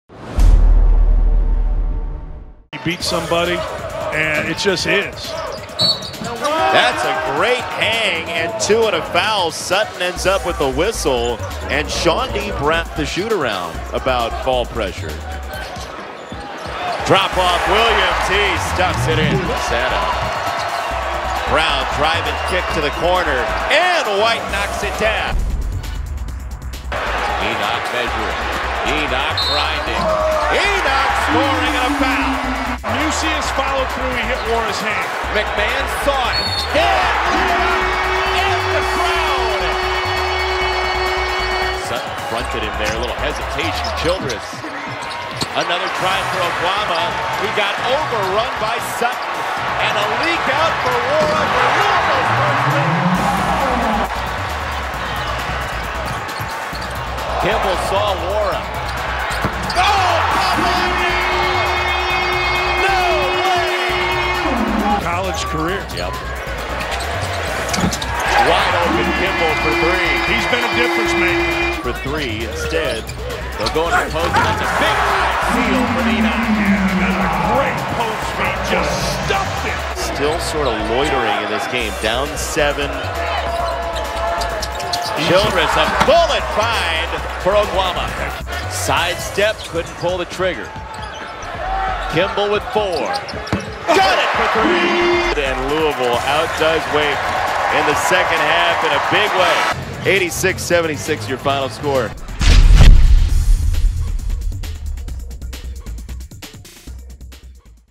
Wake Forest vs. Louisville Men's Basketball Highlights (2019-20)
Wake Forest vs. Louisville: The No. 5 Louisville Cardinals had to rally in the 2nd half for a 86-76 win over Wake Forest at home on Wednesday.  The Cardinals trailed by 12 points at halftime, but a total team effort helped them outscore the Demon Deacons 52-30 in the second half on the way to the win.  Six players scored in double figures for the Cards, led by Jordan Nwora's 21 points on 6-of-12 shooting.  Lamarr "Fresh" Kimble continuously hit big shots, scoring all 14 of his points in the second half.  Wake got off to a hot start and built a 15-point lead in the first half on 6% shooting, but the Louisville defense held the Deacs to just 30% shooting in the second half.

Thumbnail photo courtesy of:@LouisvilleMBB